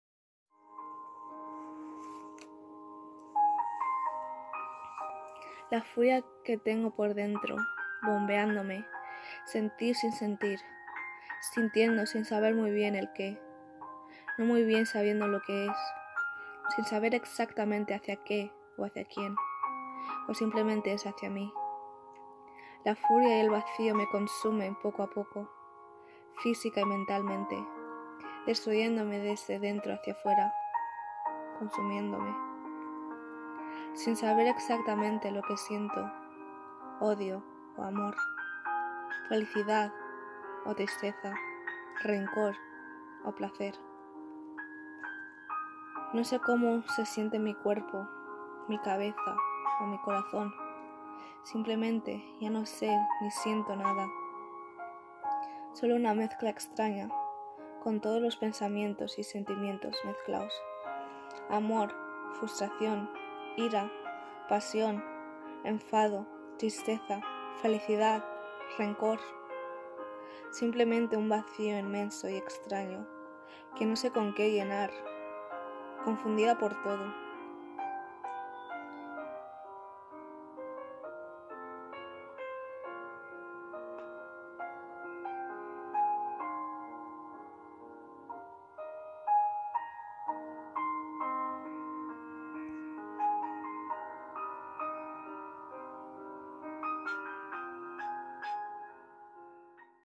Extraña sensación (Poema)
la sensación que sentimos todos en algún punto de nuestra vida, pero no temáis, se pasa solo o con ayuda hacerlo como mas a gusto os sintáis pero recordad que tendréis a alguien ahí para lo oque queráis